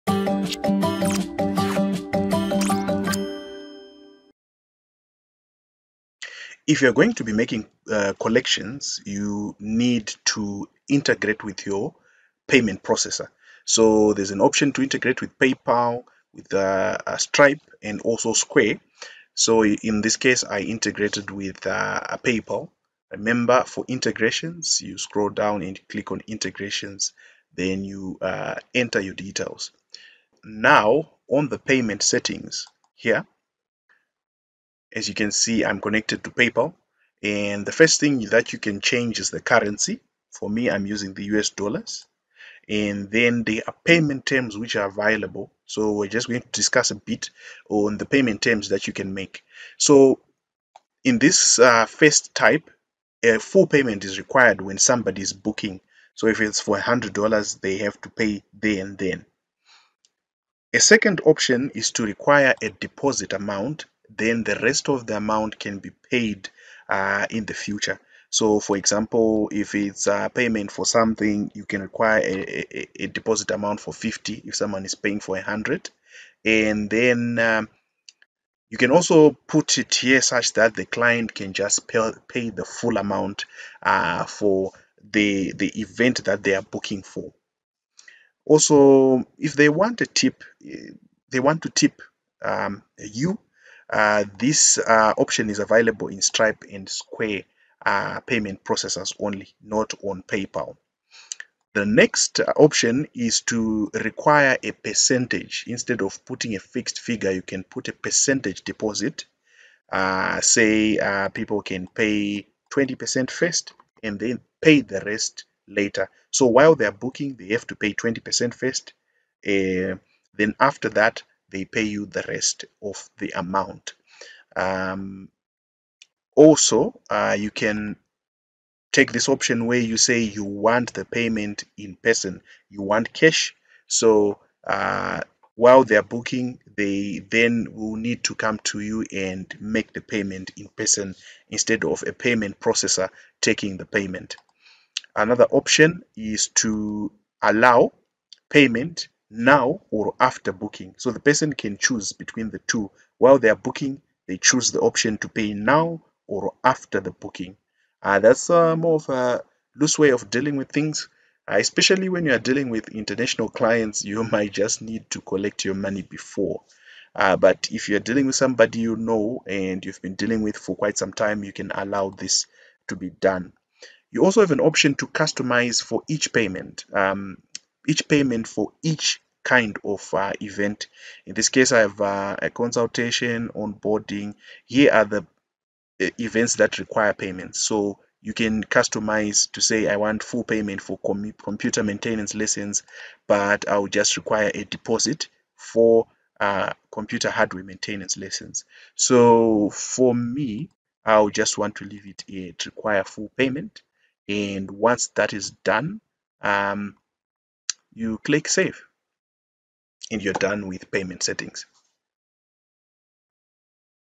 Payment Settings on Acuity Scheduling (Free Course - Part 11)
Need help setting up Acuity/Squarespace Scheduling?

Acuity Scheduling can collect payments for your services before and after bookings. This tutorial will help you see options available and ways to set them up.